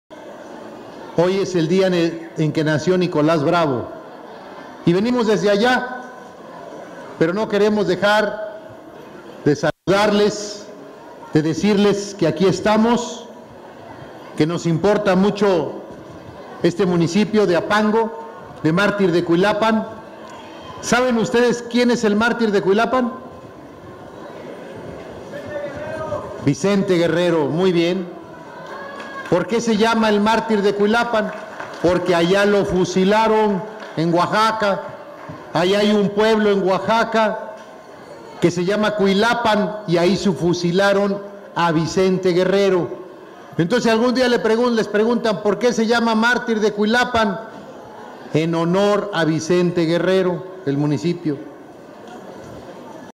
GradoCeroPress mensaje del gobernador HAF en Martir De Cuilapan en evento de entrega de apoyos
LEONARDO BRAVO (CHICHIHUALCO) y MÁRTIR DE CUILAPAN (APANGO).-

A fin de mejorar la calidad de vida de las familias de estos municipios de la zona Centro del estado, el gobernador Héctor Astudillo Flores, acompañado por su esposa Mercedes Calvo, presidenta del DIF Guerrero, entregó más de 105 millones de pesos en obras carreteras, rehabilitación de centros de salud en diversas comunidades, así como de infraestructura educativa, de vivienda como "Un cuarto más", Estufas Ecológicas y apoyos alimentarios.